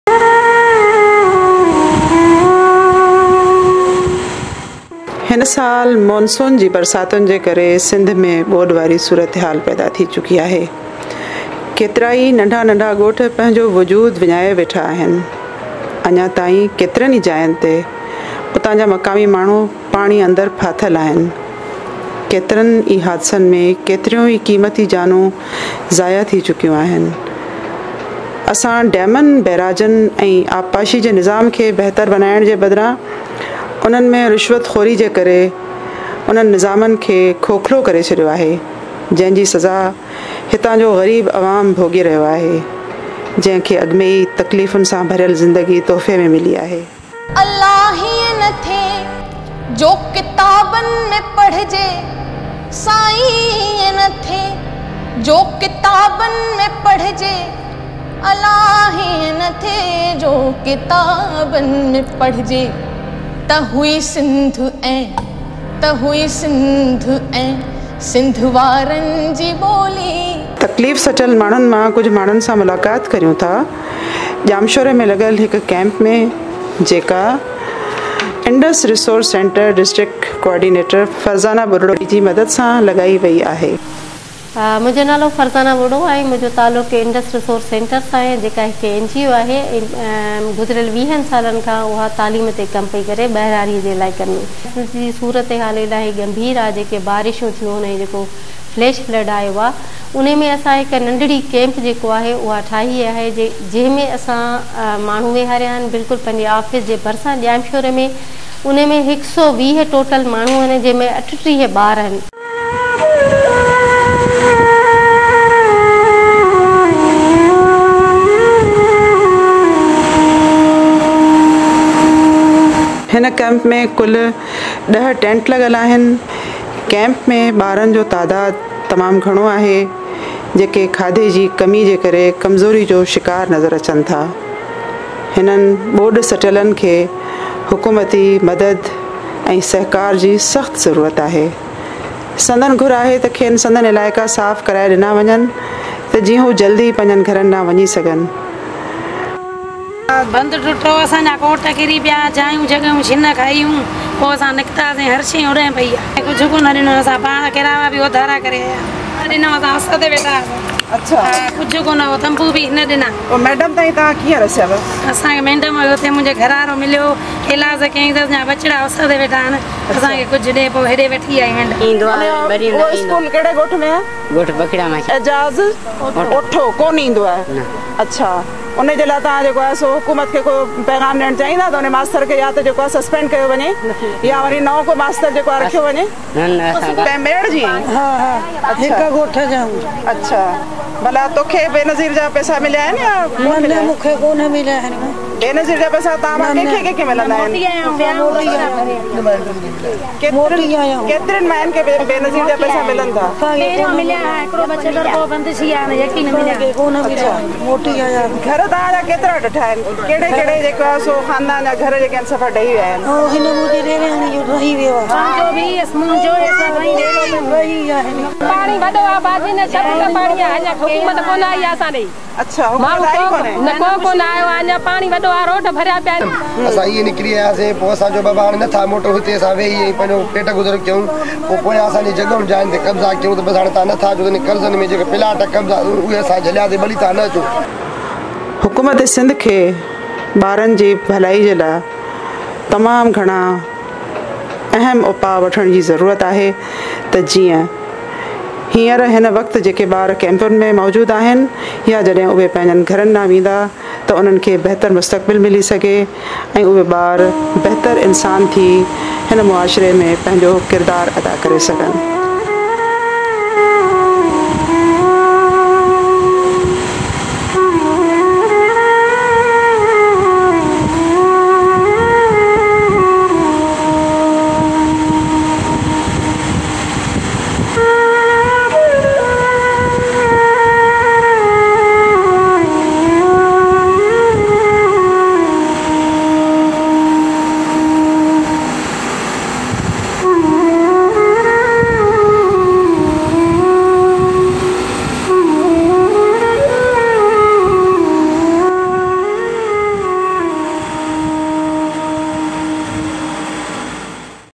Devastating Rain in Sindh and Mismanagement of Sindh Government
A Documentary about Flood affected people at District Jamshoro (IRC Camp)